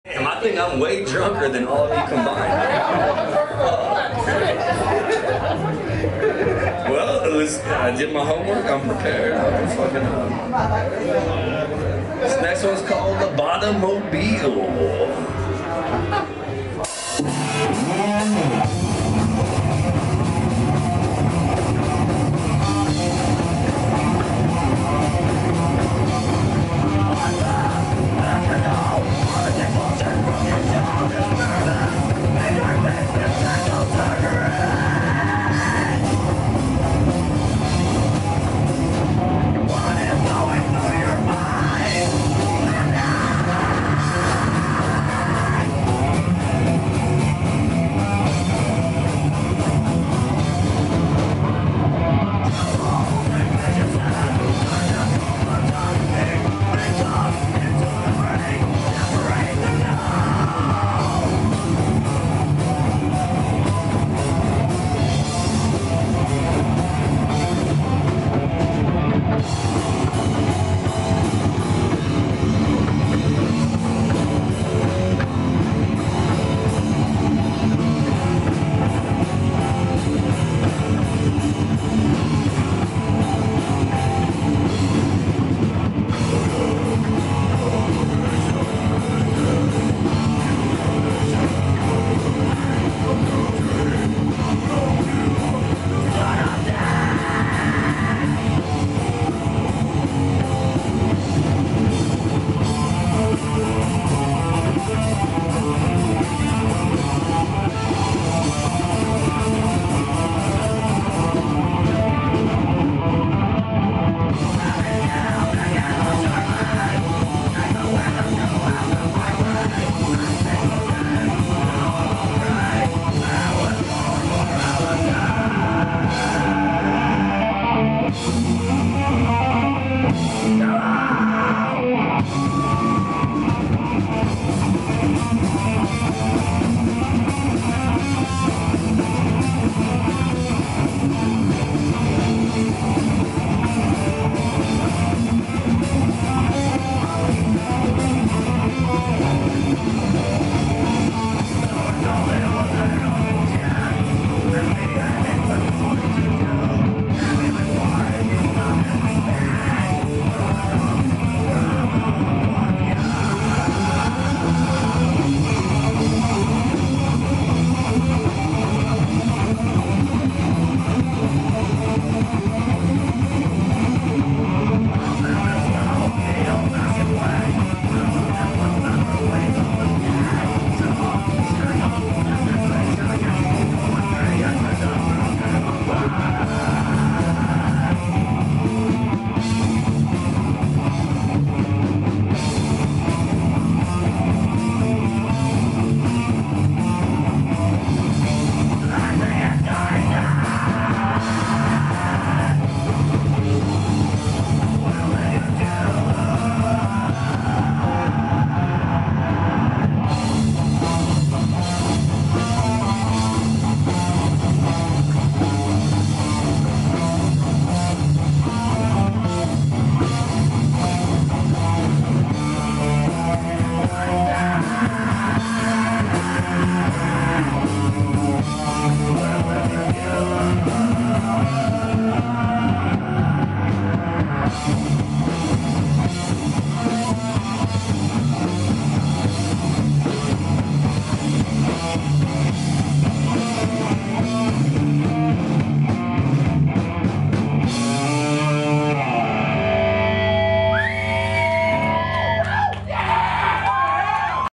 Fountain of Bile @ The Blue Lag #2
Fountain of Bile deliver the opposite of slick gutless poseur metal at the Blue Lagoon, Santa Cruz, 4th March 2017 ~ my second of three vids of the set ~ pics etc at Boris Sev facebook ~